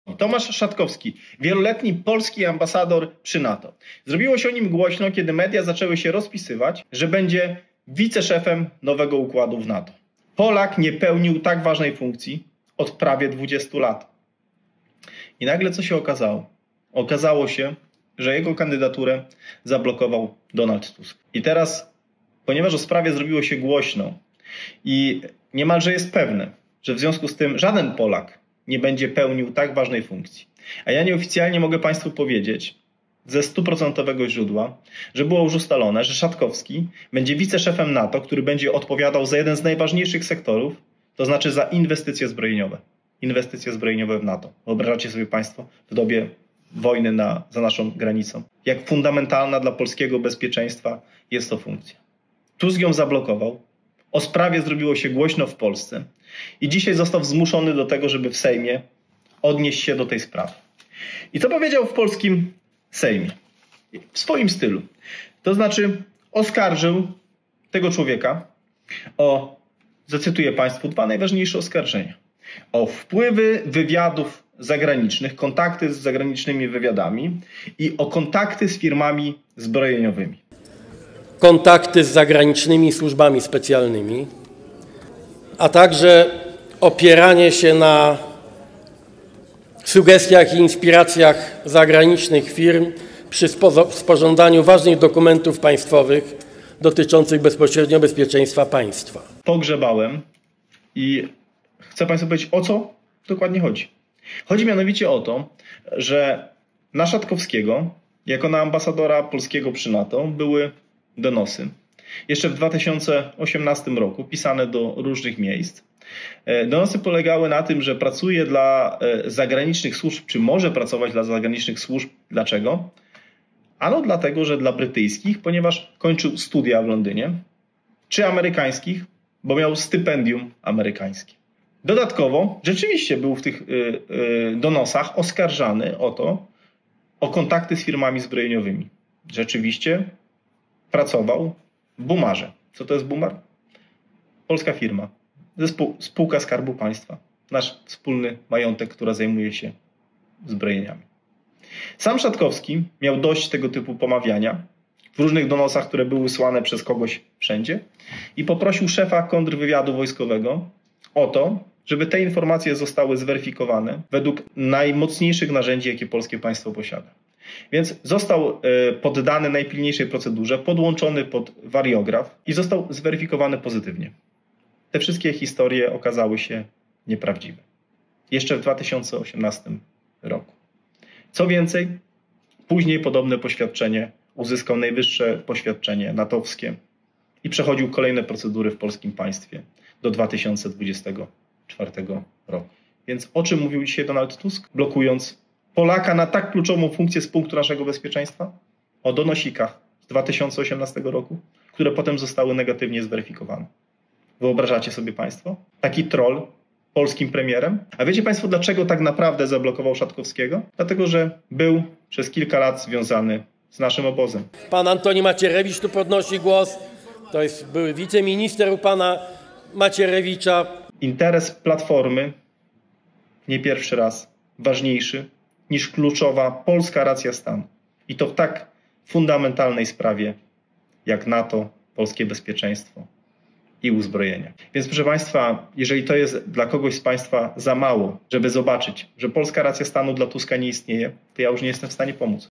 Tomasz Szatkowski oszczerczo zniszczony przez Tuska
Wieloletni polski ambasador, osoba świetnie zorientowana w sprawach NATO, ze świetnymi relacjami z USA miał zostać wiceszefem w strukturach odpowiedzialnym za zakup uzbrojenia.

Niestety na drodze stanął Tusk ze swoją proniemiecką służalczością i zniszczył kandydaturę Polaka na tak ważne stanowisko - poznajcie tę historię!